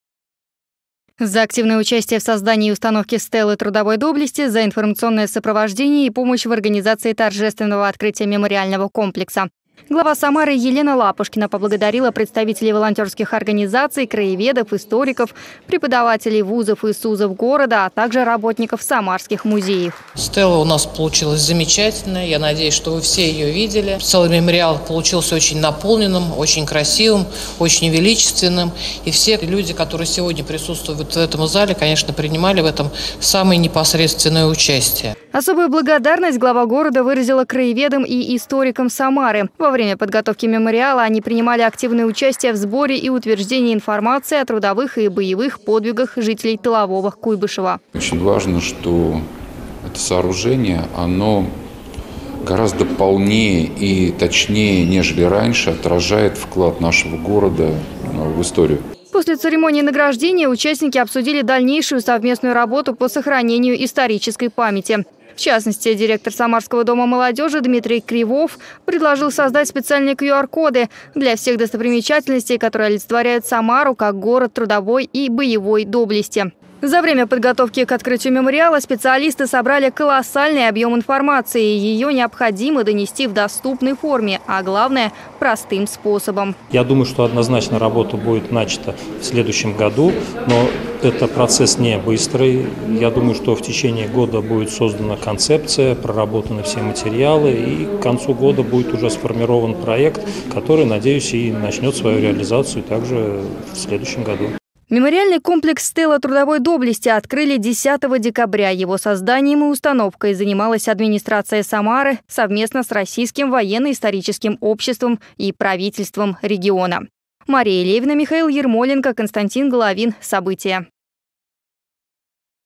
Елена Лапушкина поблагодарила участников создания комплекса "Самара — город трудовой доблести"
В Самаре наградили жителей, которые участвовали в разработке и установке стелы "Город трудовой доблести". Архитектурный комплекс появился на Аллее трудовой славы в Кировском районе, как олицетворение подвига тылового Куйбышева, который в годы Великой Отечественной войны стал запасной столицей. Подробнее расскажем в сюжете.